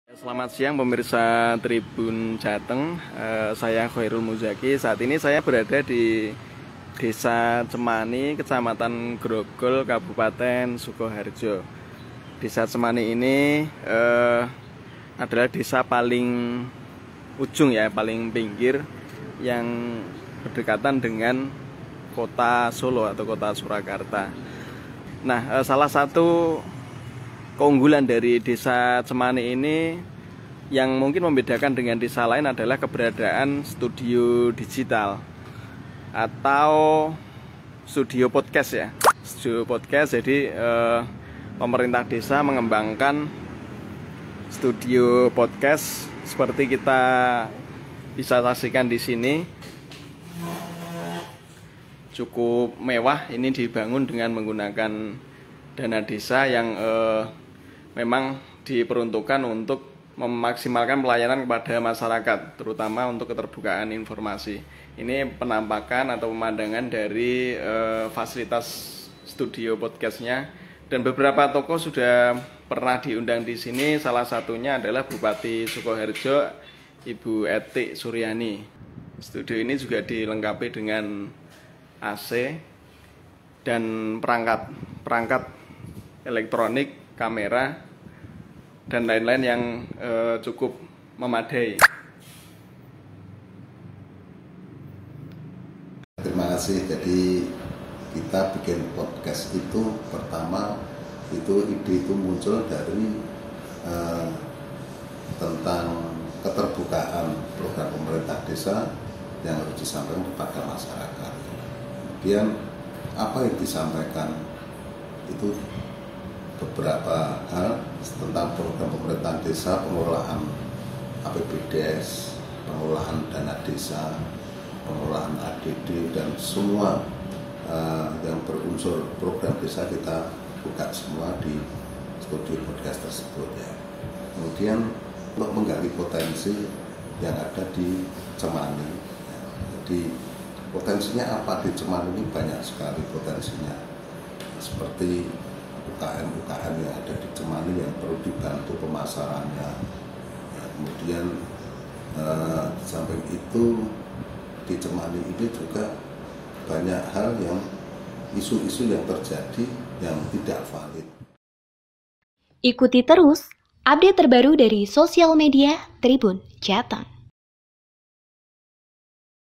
Megahnya Studio Podcast Desa Cemani Sukoharjo Tingkatkan Akses Informasi
Studio Digital Pemerintah Desa Cemani berada di pojok depan Balai Desa Cemani Kec Grogol, Kab Sukoharjo.

Pemdes Cemani sengaja membangun studio digital untuk meningkatkan pelayanan ke masyarkat, terutama akses informasi.

Beberapa hari sekali, Pemdes Cemani mengagendakan talk show untuk mengupas tema tertentu.

Pemdes akan lebih mudah menyampaikan informasi positif kepada masyarakat, termasuk terkait kebijakan pemerintahan.